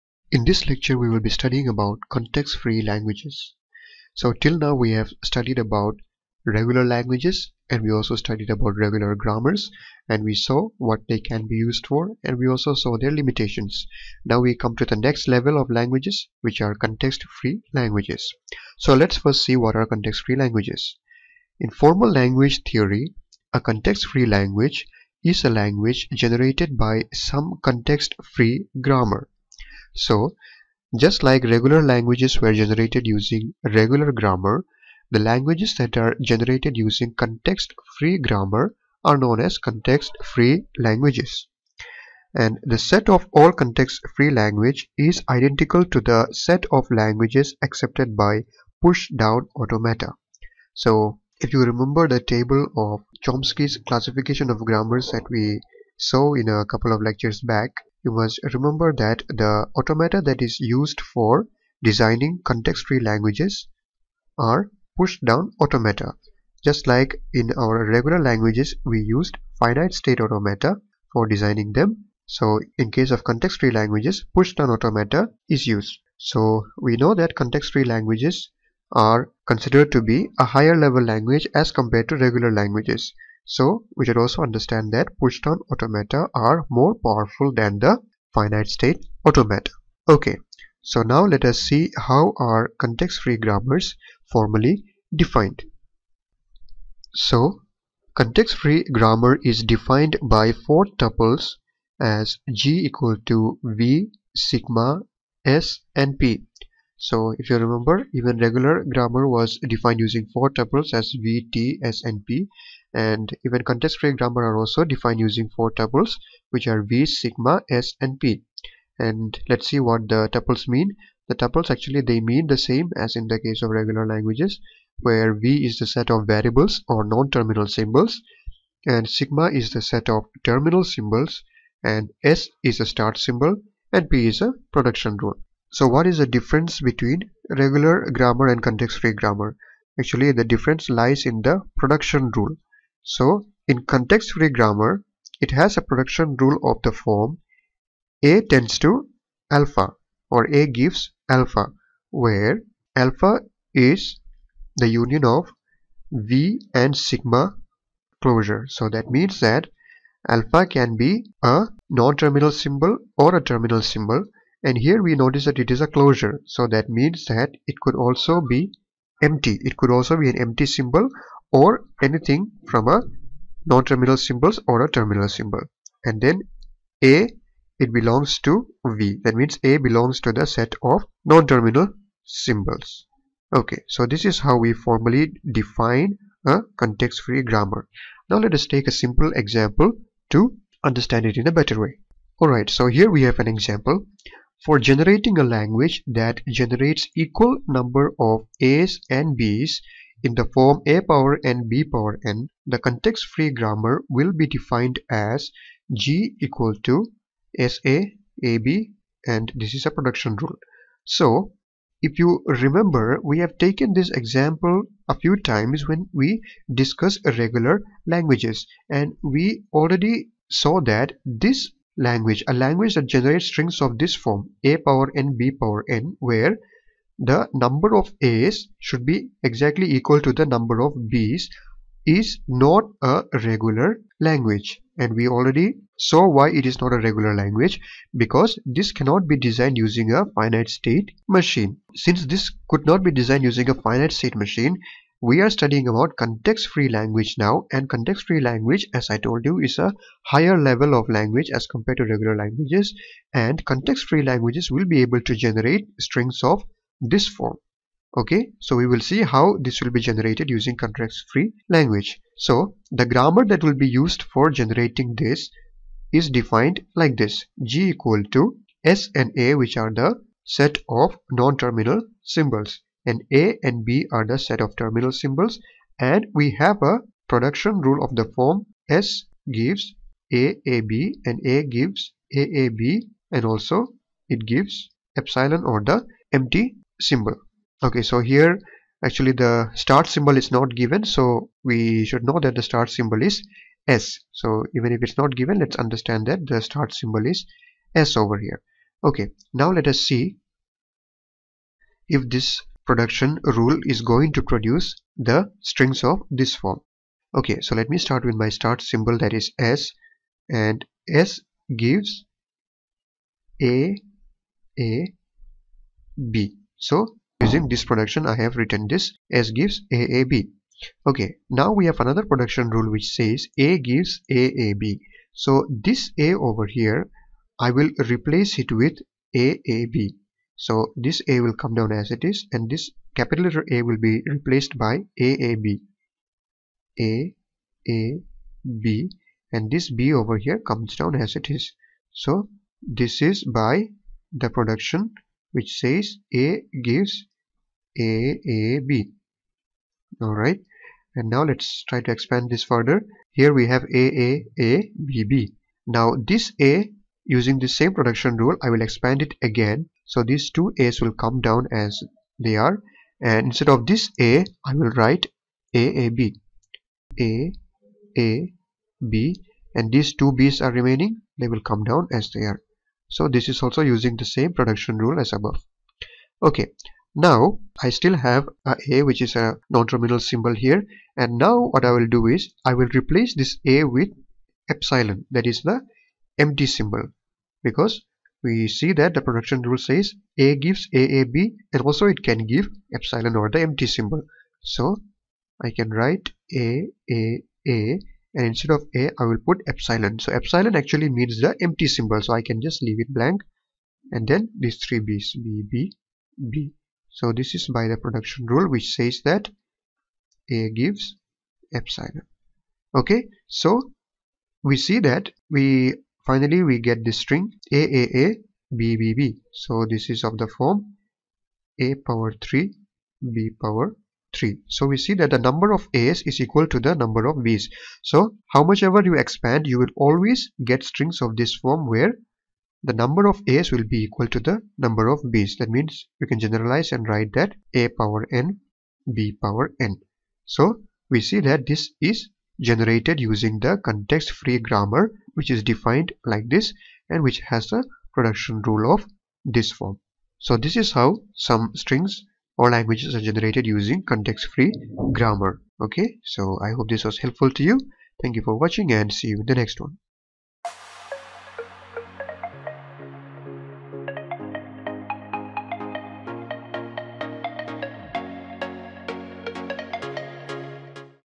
Context Free Grammar & Context Free Language
TOC: Context Free Language
Topics discussed:
1. Context Free Language
2. Context Free Grammar
3. Example of CFL generated using Context Free Grammar.